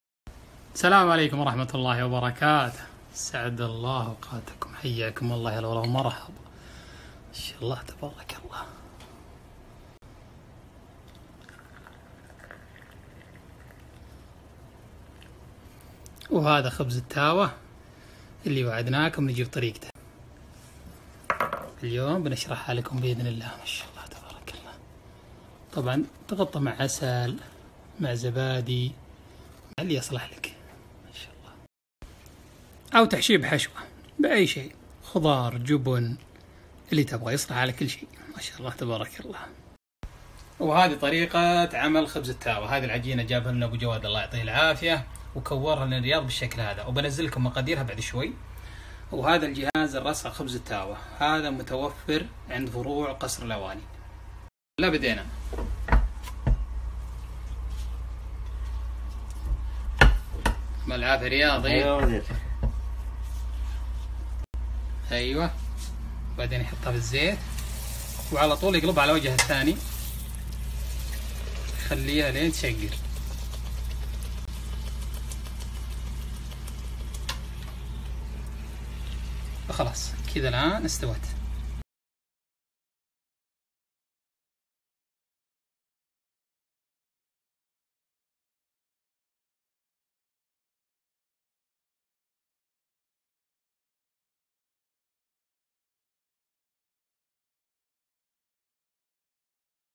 خبر التاوة طريقة عمله لذيذ و موفيد
مكونات تحضير
ثلاث اكواب دقيق بر
كوب دقيق ابيض
فنجال سكر
ملعقة خميره فورية
رشة ملح
ثم تضاف هذه المكونات على كوبين ماء دافئ وتعجن ويكون قوام العجينة ثقيل وتترك العجينة مدة ساعه الي ان تتخمر
ثم تدهن اليدين بزيت وتكور العجينة كور صغيره  ثم تضغط بالة المطازيز ثم توضع على زيت القلي مباشرة بعد ارتفاع درجة حرارته وبالهناء والعافيه